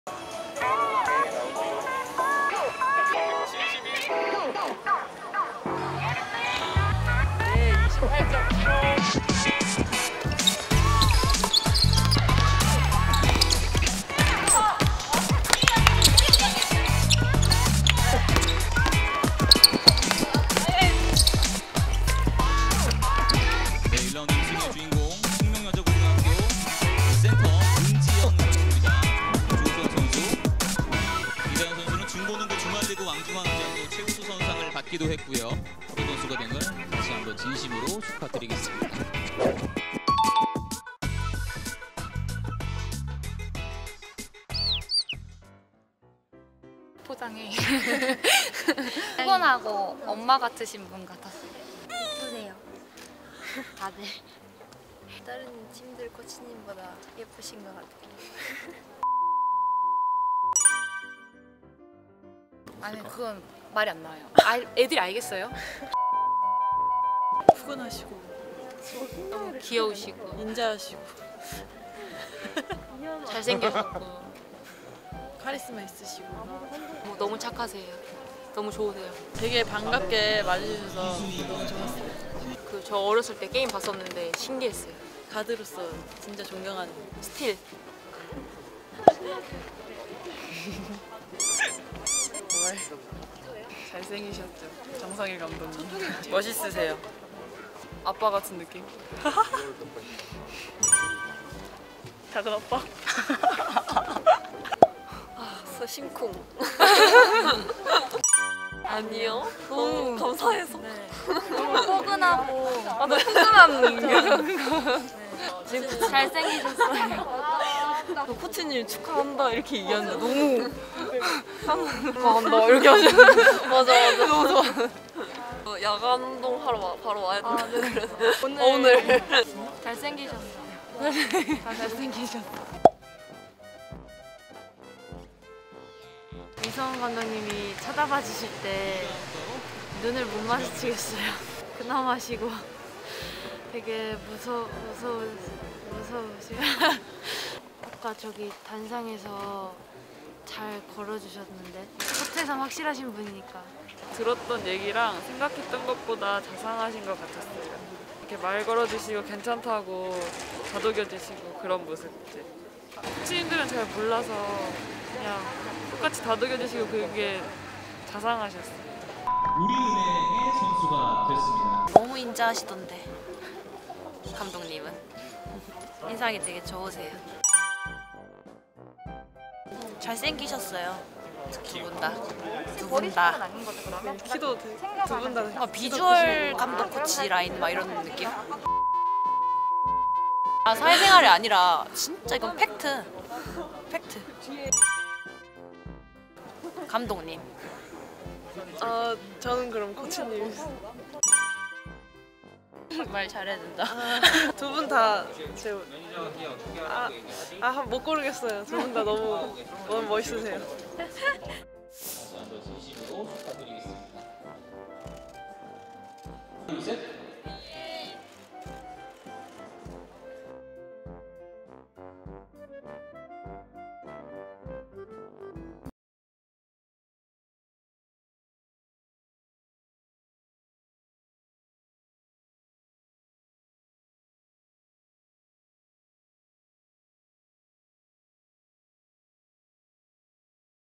WKBL 막냉이들의 사회생활을 ARABOZA!! 막내들의 아부력은?
총 27명이 참가해 14명이 선발된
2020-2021 WKBL 신입선수 선발회!

취업 후에 가장 중요한 건 사회생활이겠죠?

그래서 여농티비가 WKBL 막냉이들에게
사회생활 테스트를 해봤습니다!

심상치 않은 막냉이들의 아부력 ㄷㄷ
영상으로 확인하시죠